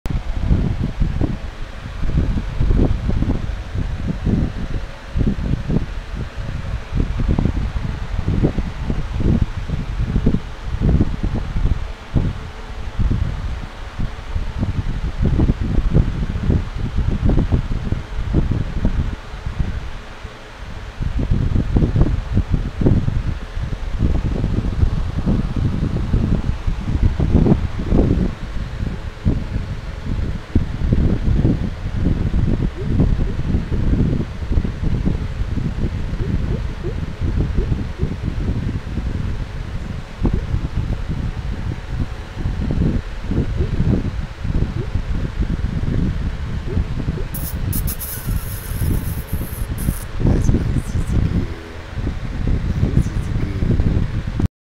Special Effects-Cece, the pitbull, thunderstorm dream
Our beautiful pittie, Cece, was dreaming. I assumed it was about a calm, relaxing thunderstorm! :)